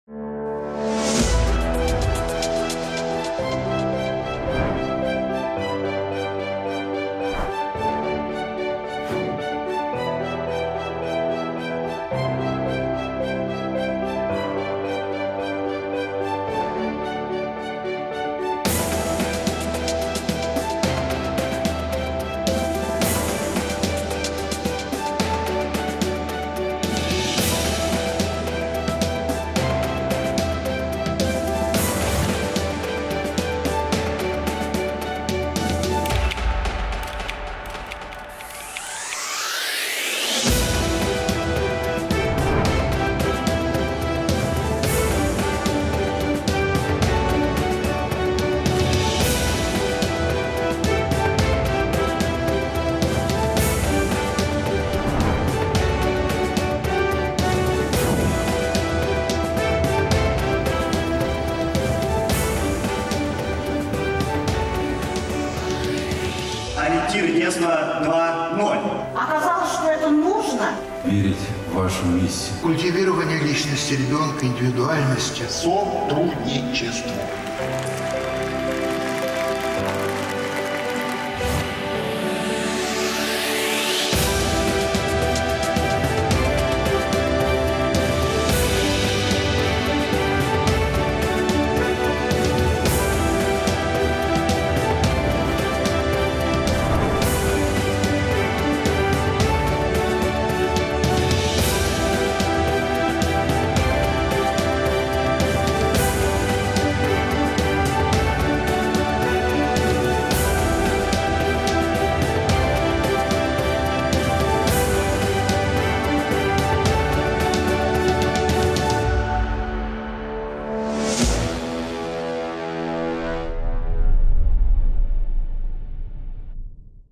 Ориентиры детства 2.0. Всероссийский форум работников дошкольного образования
26 августа 2019 года в Crocus City Hall (г. Москва) прошел Всероссийский форум работников дошкольного образования «Ориентиры детства 2.0» – крупнейший в мире форум для воспитателей, собравший 6000 педагогов со всей России.  Спикеры форума — ведущие эксперты в образовании, психологи, педагоги-новаторы. Современные педагоги все больше интересуются новыми методиками образования и хотят развивать профессиональные навыки: научиться эффективно взаимодействовать с коллегами, детьми и их родителями; больше успевать; имеют личностные интересы, планы, участвуют в профессиональных конкурсах, конференциях.

«Ориентиры детства 2.0» — это новая программа, новые спикеры, новые источники вдохновения и векторы развития. Миссия форума — рассказать о возможностях для профессионального и личностного роста педагога.

Форум помог педагогам выстроить индивидуальную траекторию развития собственных компетенций, обрести поддержку коллег, покажет, как и чем дополнить свой профессиональный опыт, дал энергию для работы.

Организаторами форума выступили Международная педагогическая академия дошкольного образования, Ассоциация развития качества дошкольного образования, Общественная палата РФ, Российское общество «Знание». Мероприятие прошло при поддержке Министерства просвещения Российской Федерации, Фонда президентских грантов, Российской академии образования, Комиссии РФ по делам ЮНЕСКО.